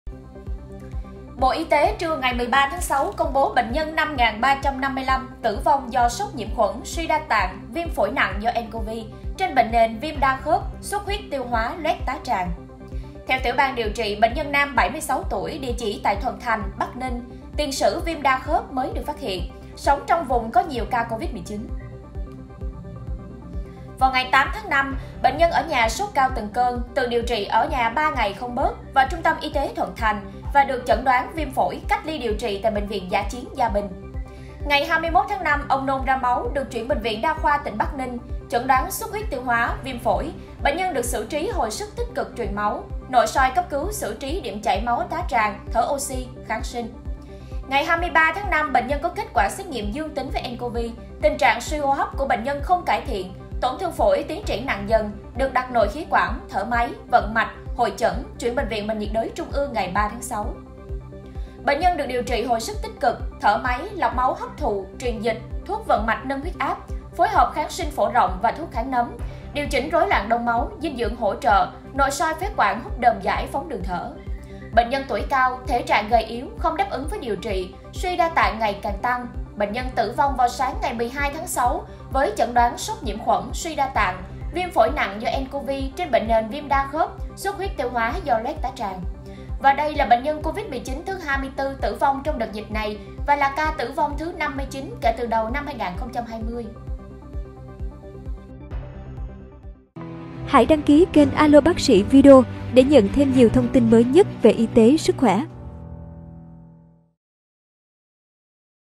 Bệnh nhân COVID-19 thứ 59 t.ử v.o.n.g| Video AloBacsi
Bộ Y tế trưa 13/6 công bố "bệnh nhân 5355" t.ử v.o.n.g do sốc nhiễm khuẩn, suy đa tạng, viêm phổi nặng do nCoV, trên bệnh nền viêm đa khớp, xuất huyết tiêu hóa loét tá tràng.

MỌI GÓP Ý XIN GỬI VỀ:
Hợp tác nội dung:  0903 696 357